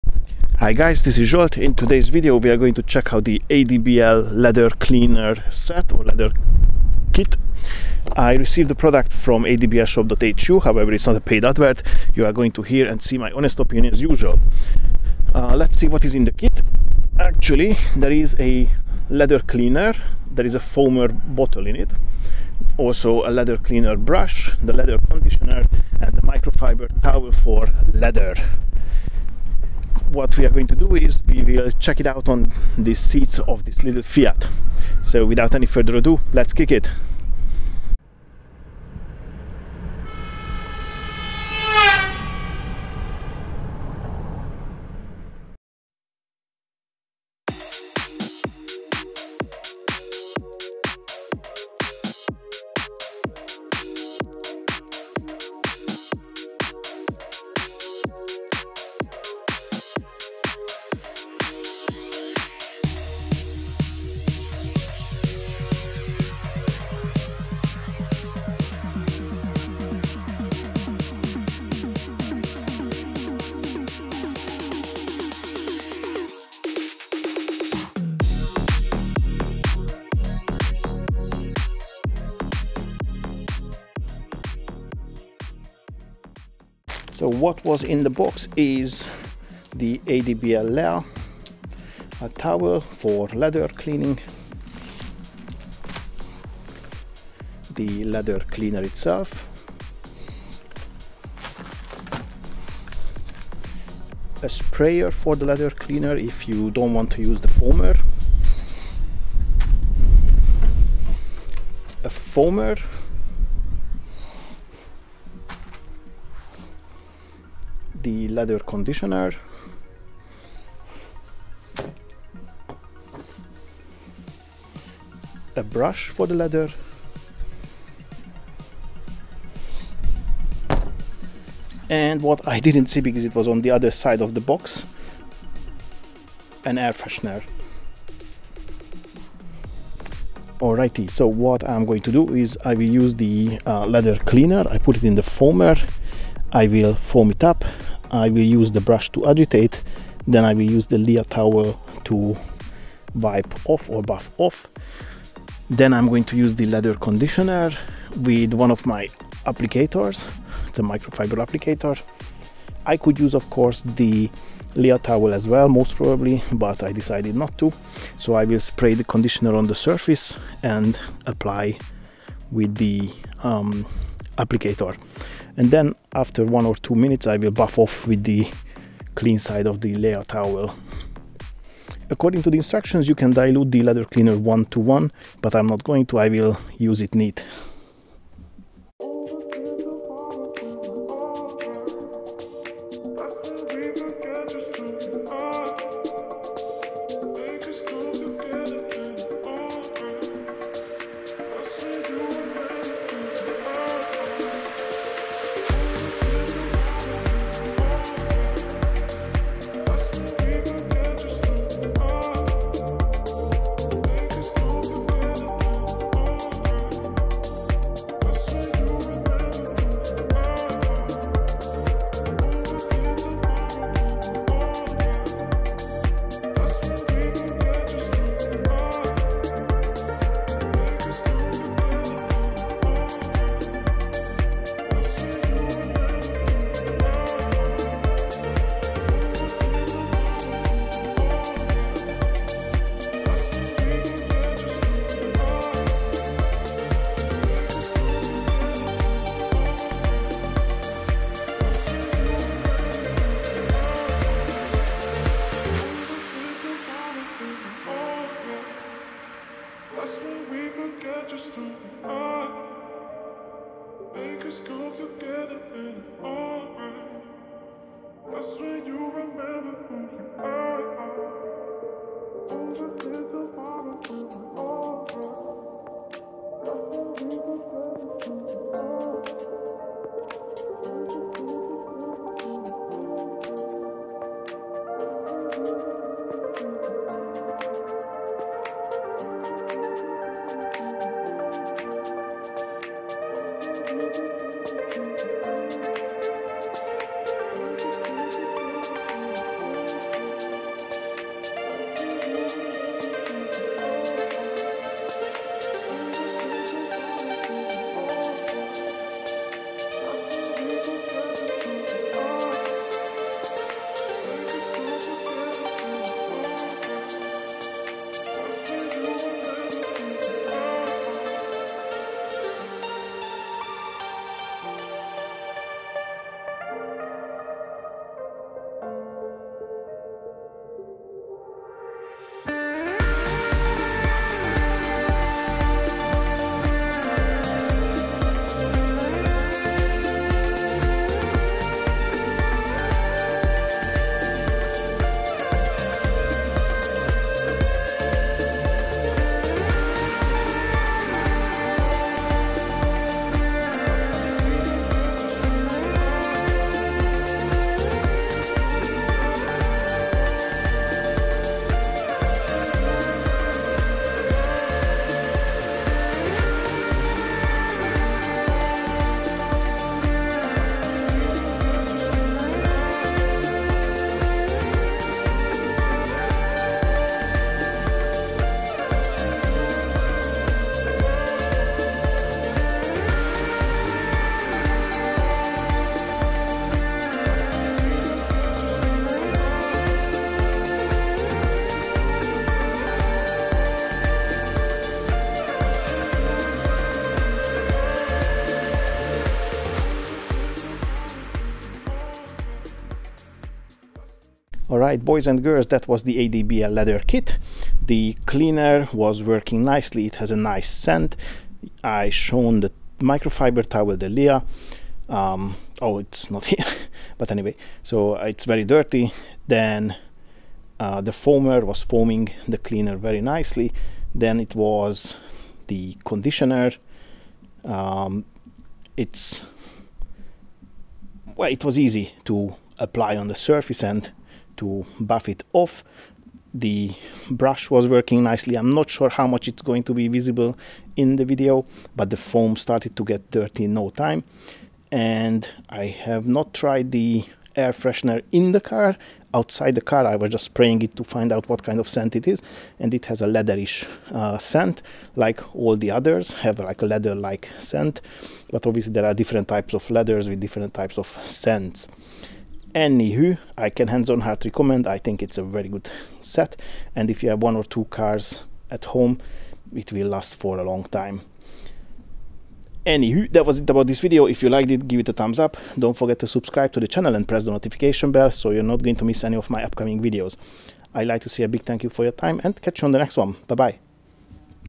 ADBL Leather Kit test
This video is not a paid advert, however I received the product free of charge in order the be able to show you. It is about the ADBL Leather Kit's test.

Further information (in Hungarian, or you could search in your local area)

Sonax car care products: sonax.hu